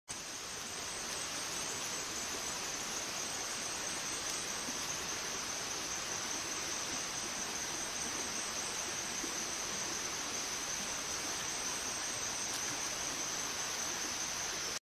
Northern Naked tailed Armadillo crossing a trail - Soberania National Park Panama
filmed at Plantation Road Trail - Soberania National Park